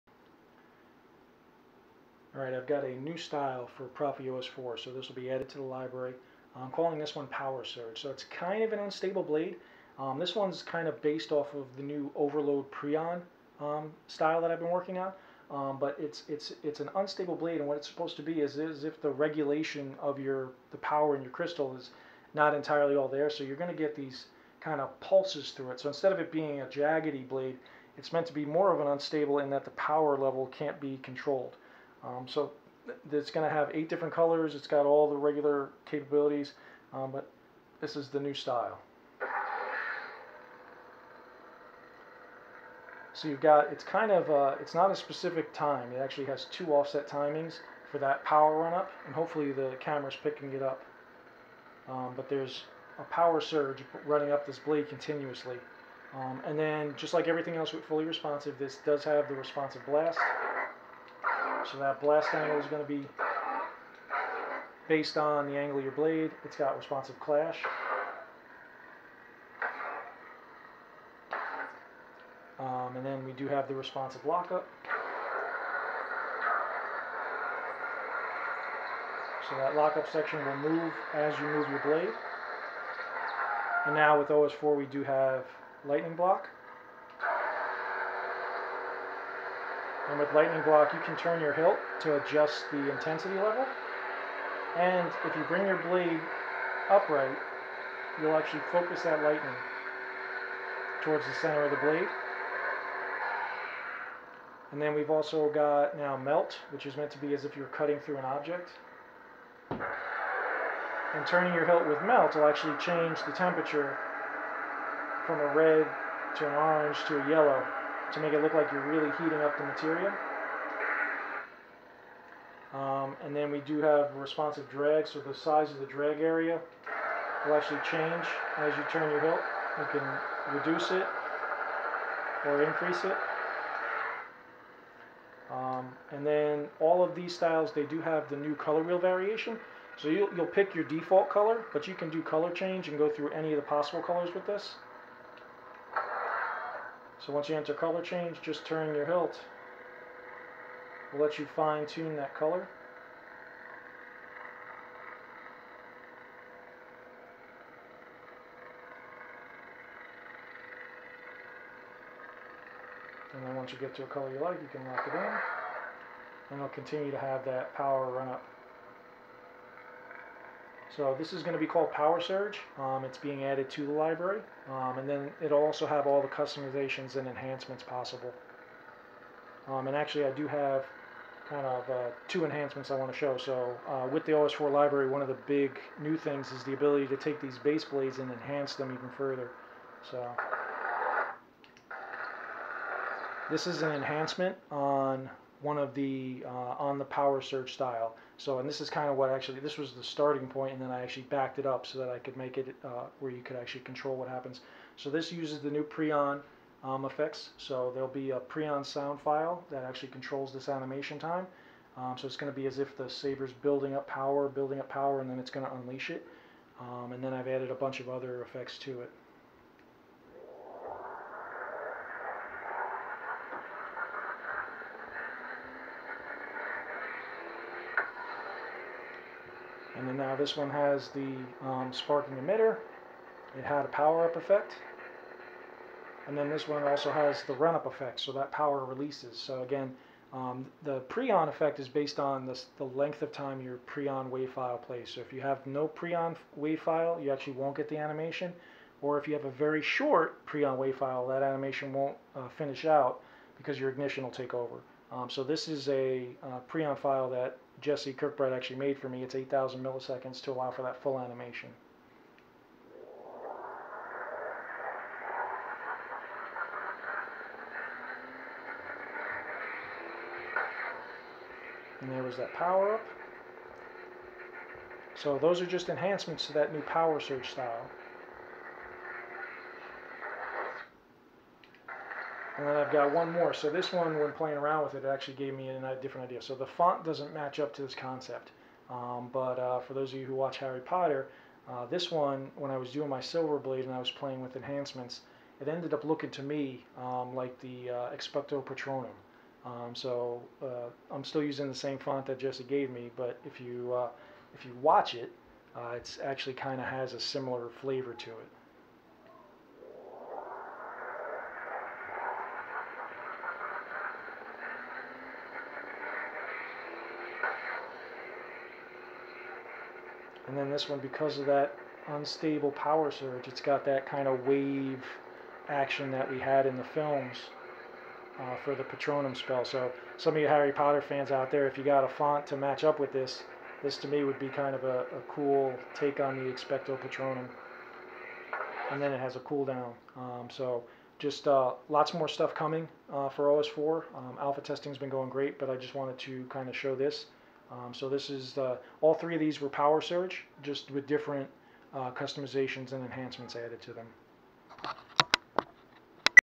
Fully Responsive "Power Surge" Style for ProffieOS 4 Lightsabers
The new "Power Surge" Unstable neopixel lightsaber style is meant to mimic a blade with a faulty regulator that cannot keep control of the power of the kyber crystal. This style features a continuous Power Surge up the blade from the crystal causing a different type of unstable effect. This style wokrs veyr well with the new "Overload" preon effect (if you have preon sound to support). In addition, this style features the new ColorWheel variation that will change the color of the main blade without affecting the other effect colors.

Fully Responsive Styles included:

Responsive Blast = instead of a randomized location, the blast location is now based on the angle of your blade to create a more realistic and immersive blast deflection effect, it features transitions in and out that the user can choose and modify

Responsive Clash = instead of a randomize clash location, the clash effect will mirror the Responsive LocalizedLockup location to help with choreography and realism. The location of the Clash effect is based on the angle of your blade and allows you to line up clashes for the Responsive Lockup

Responsive Lockup = rather than an animated effect I would typically use to mimic blades sliding against each other in a struggle, LocalizedLockup now moves based on the angle of your blade allowing for a more realistic effect as you move towards yourself defensively the lockup slides down the blade and as you move your blade towards your opponent it will slide up the blade to give you a real struggle effect, it features a quick flash transition in like in the films and a power absorption transition out as the blade restores itself.

Responsive LightningBlock = this hybrid effect features a Force Lightning effect that dances along the blade, you can control the intensity of the attack by turning your hilt to have it be more intense of less intense. In addition, it uses Responsive control to "focus" the attack as you move your blade to the straight up position to give you a real immersion into the effect. It features a large initial blast transition in and a power absorption transition out to add to the realism.

Responsive Melt = based on the scene from EPI when Qui Gon is cutting through the blast door, melt mimics a surface being heated and melted, you control the "temperature" and intensity of the melt by turning your hilt it will heat the material from a Red to very hot Orange/Yellow and back based on turning of your hilt, it features a wipe in and wipe out transition to mimic the end of the blade entering and exiting the surface.

Responsive Drag = the tip Drag now responds to the turning of the hilt to increase or decrease the depth of the drag area. it features a burst and fade out transition

Responsive Stab = the Stab effect will change in size based on the angle of the blade, if you Stab downwards the effect will be a larger, deeper depth, it features a wipe in and wipe out transition